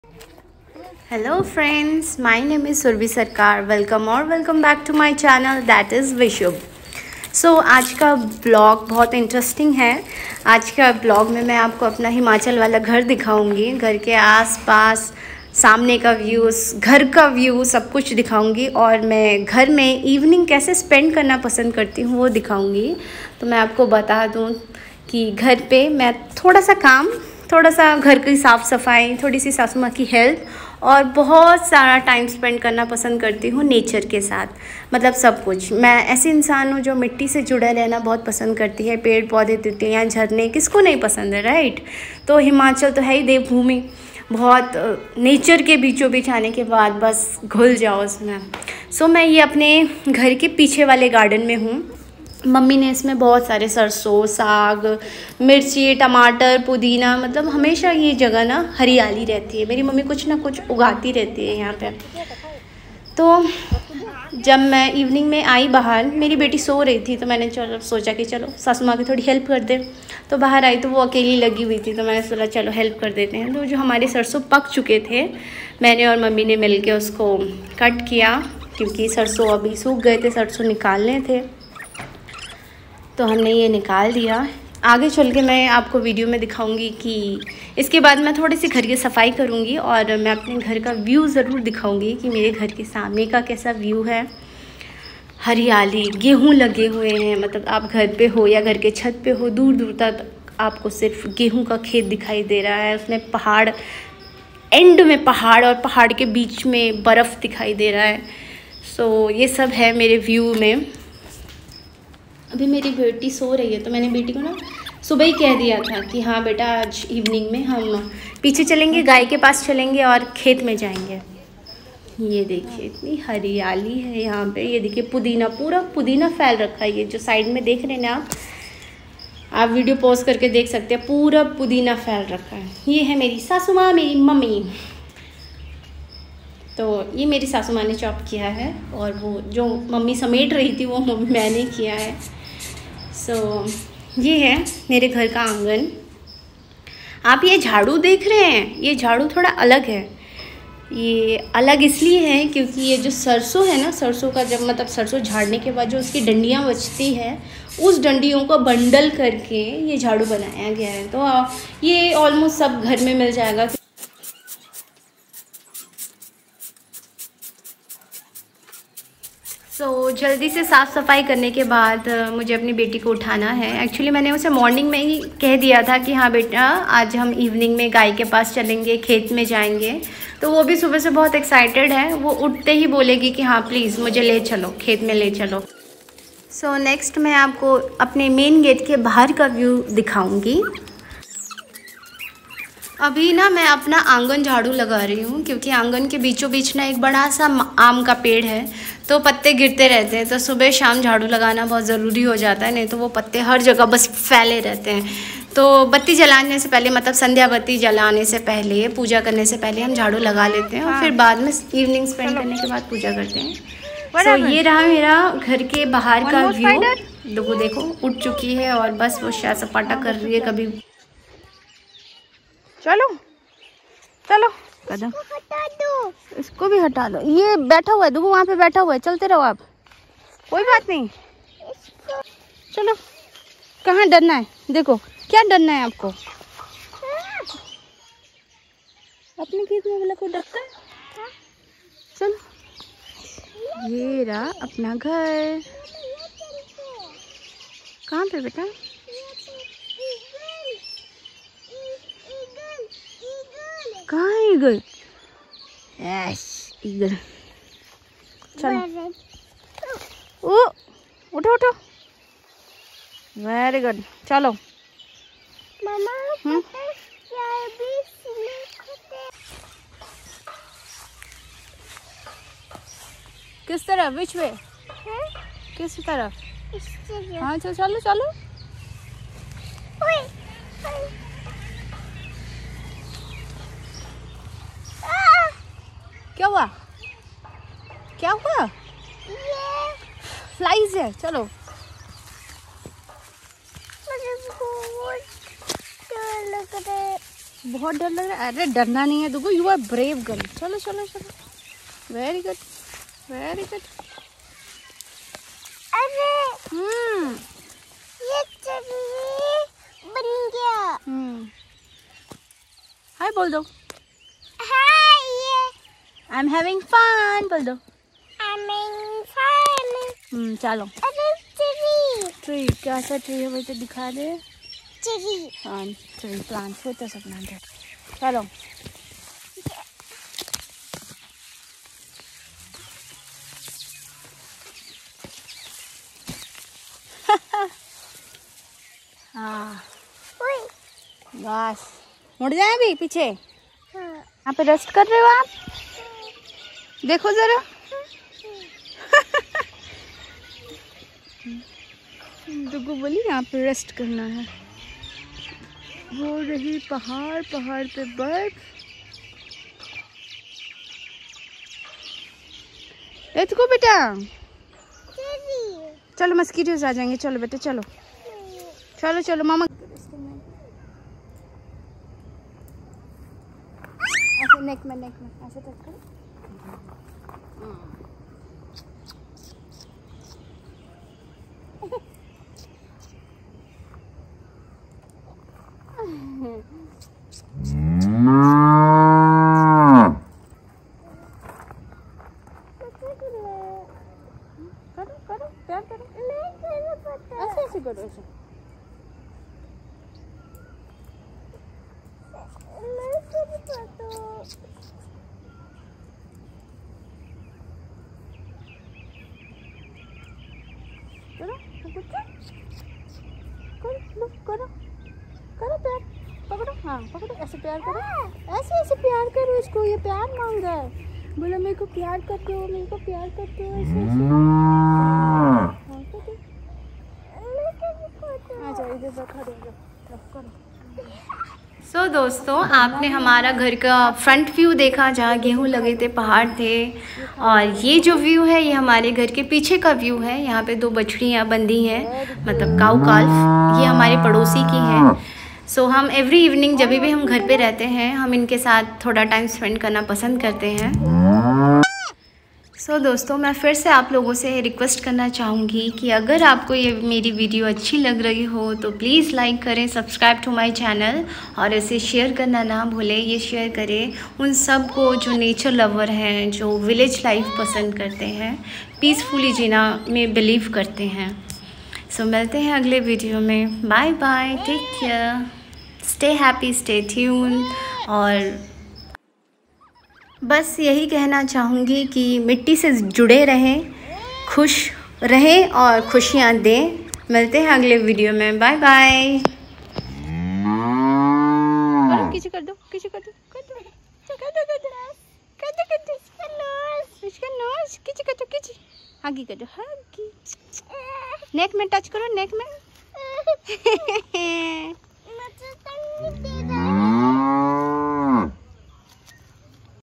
Himachal village life | Kangra village | Dharamshala | Evening routine in village
Spending some quality time with nature and animals is my most wanted wish…
My village is located very closely with Dharamshala a world renounced tourist place with tons of natural beauty and peace